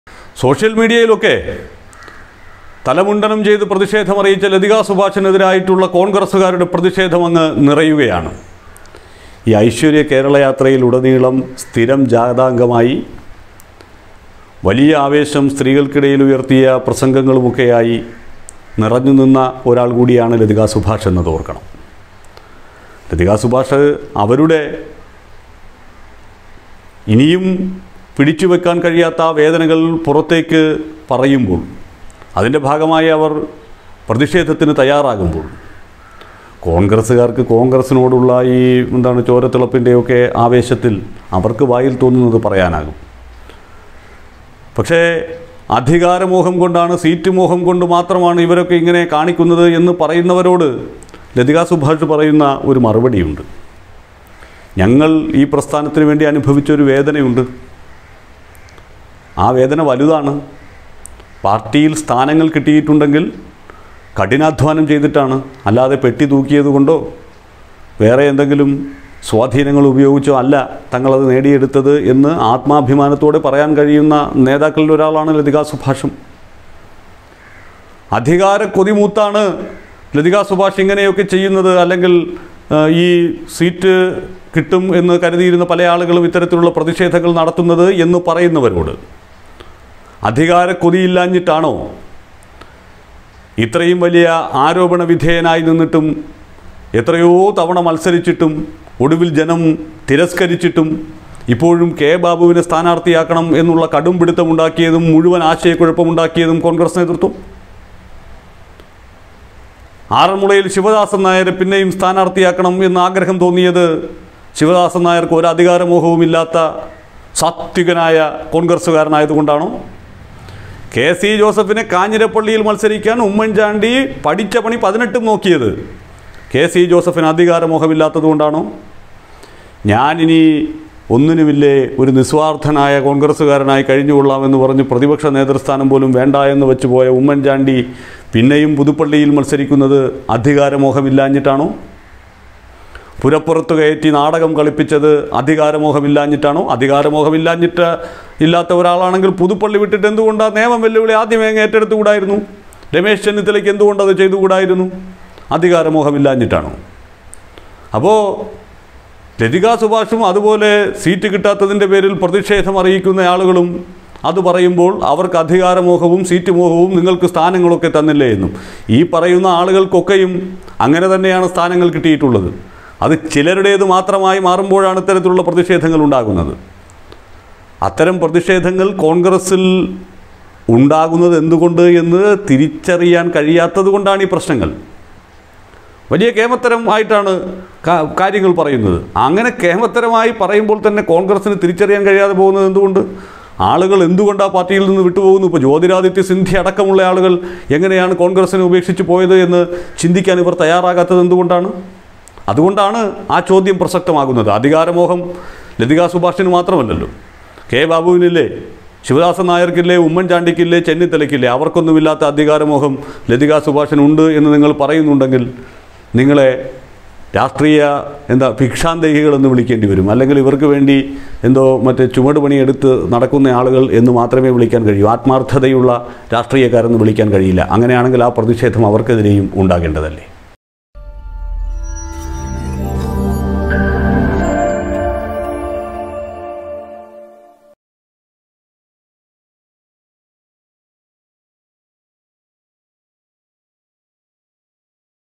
നായകനെ നേരിടാതെ പിന്നെങ്ങനെ പോരാട്ടത്തിൽ ജയിക്കും
പിണറായിക്കെതിരേ
മത്സരിക്കാൻ ആളില്ലാത്ത കോൺഗ്രസോ